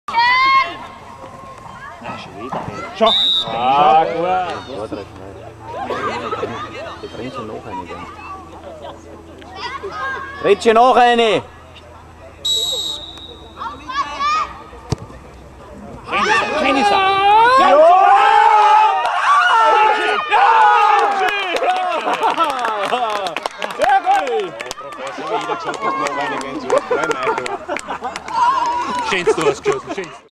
2011-09-28_Fernitz-U12 - Maria Trost 1zu0.mpg
Freistoß Florian - Richard schließt ab